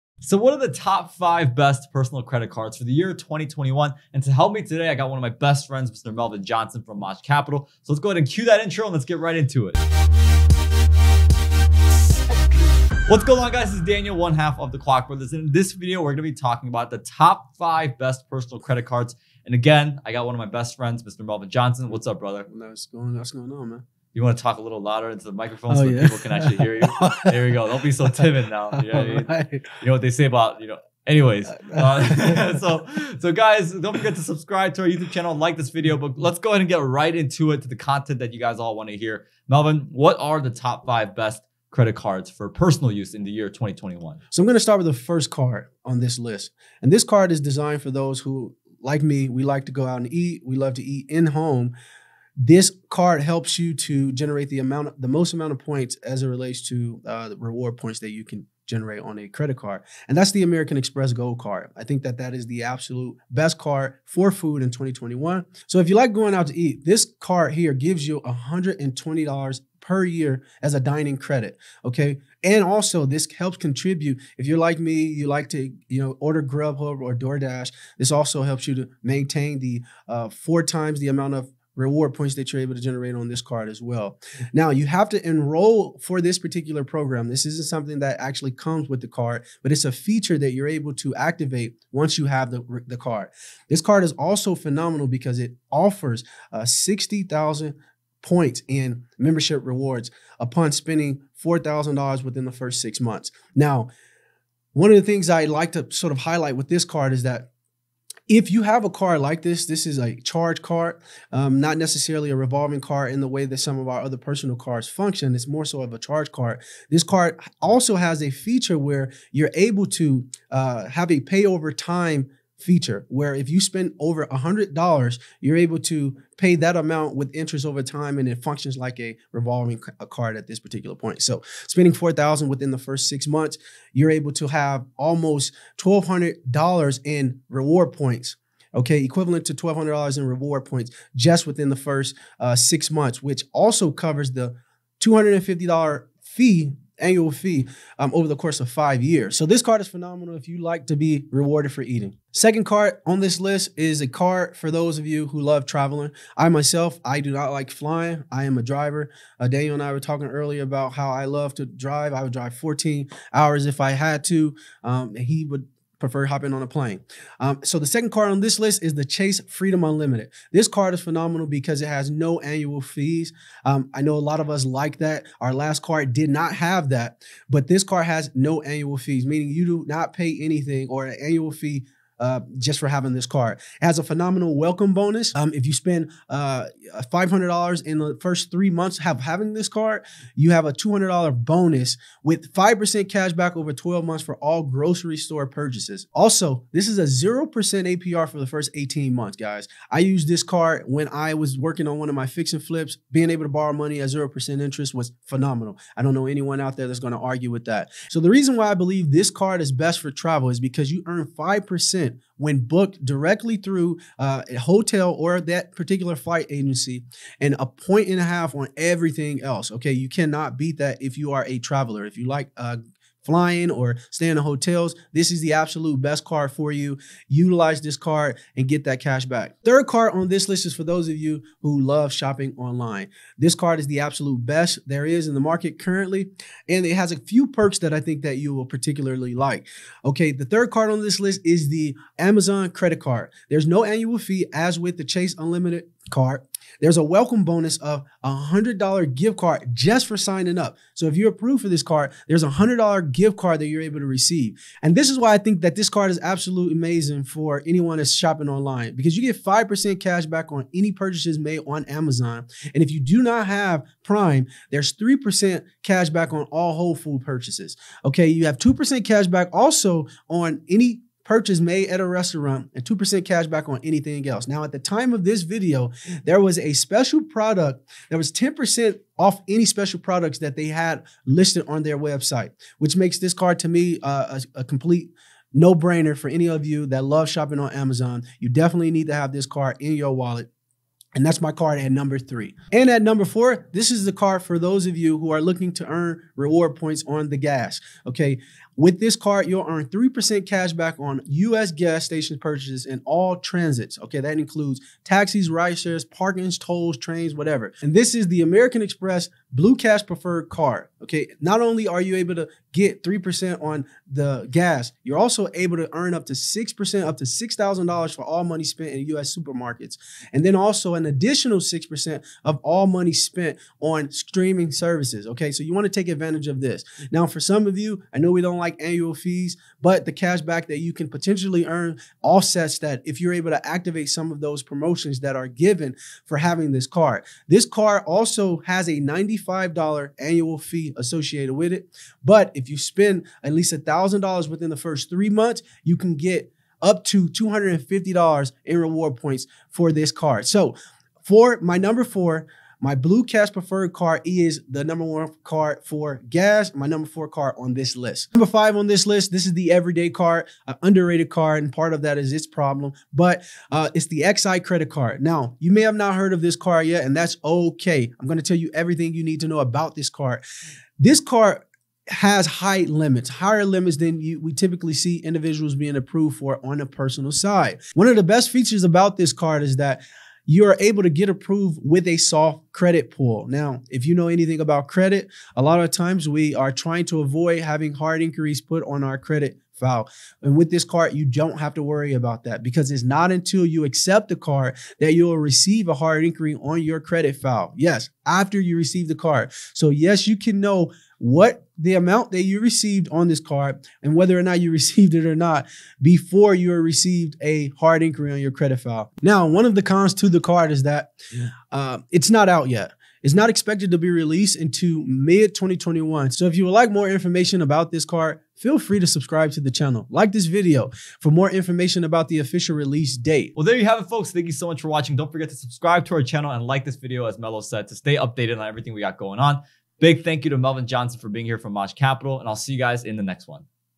Best Credit Cards in 2021
2021 Best Credit Cards That PAY YOU BACK, in this video we bring back our friend Melvin Johnson of Maaj Capital to talk about the BEST personal credit cards to look into getting for 2021.  Now with these credit cards, keep in mind, that these are NOT starter credit cards or secured cards.  These credit cards are intended for people who have already established credit with good to excellent credit.  When used correctly, credit cards can provide opportunities for cash back, travel rewards, and other rewards for using credit cards for everyday purchases.  American Express, Chase, Amazon credit, and more, are exceptional companies with great rewards for those who use them correctly!

0:00 Intro
0:44 1st 2021 Best Credit Card
2:32 2nd 2021 Best Credit Card
3:50 3rd 2021 Best Credit Card
4:52 4th 2021 Best Credit Card
5:59 5th 2021 Best Credit Card

1. American Express Gold Card
The American Express Gold Card is great for food, grocery shopping, restaurants, and food delivery apps like GrubHub or Door Dash.  With the American Express Gold card, you'll be looking at 4x points at supermarkets, 4x points at Restaurants (including take-out).  $120 dining credit for select food services and restaurants.

2. Chase Freedom Unlimited Card
The Chase Freedom Unlimited card is great for those looking to get rewards from traveling.  This card is phenomenal because there are no annual fees.  The Chase Freedom Unlimited card has a welcome bonus if you use $500 within the first 3 months, you'll receive a $200 bonus.  With 5% cashback on grocery store purchases, gives you great incentives to put that food on this card. And with a 0% APR for the first 18 months, the Chase Freedom Unlimited card is great if you are looking to possibly use it for a business expense or for your next house flip! Also, this card is great for travel because you will get 5% back when booking directly with the hotel/airline!

3. Amazon Credit Card
The Amazon Credit card is great for those who LOVE to shop online.  There is no annual fee AND a $100 gift card JUST FOR SIGNING UP!  The Amazon Credit card will also provide 5% cashback on ALL Amazon purchases and 3% cashback at Whole Foods!  And then 2% cashback on any purchase that goes onto the Amazon Credit Card.

4. American Express Blue Cash Prefered Card
Now for those who are looking to get some points from the gas pump, the American Express Blue Cash Prefered card is meant for you.  With this card, you will receive 3% cashback at the gas pump, AND on any rideshare, trains, or any kind of transit.  There is an annual fee for this card, but the reward potential alone will offset that cost.

5. x1 Credit Card
The x1 Credit Card should be used for everyday purchases, and the x1 Credit Card has high limits, higher than most other cards pending approval.  One of the best things about the x1 Credit Card is that you can be approved without a hard inquiry on your credit report.  So this gives you the chance to take a look at your potential approval amount and be able to decide whether you'll take it or not without a hard pull.  But keep in mind, once approved and accepted, x1 Credit Card will put the hard inquiry once an agreement is made.

📡 PROTECT YOUR ONLINE DATA AND ACTIVITY WHILE CLOSING DEALS AND GENERATING NEW LEADS WITH NORDVPN: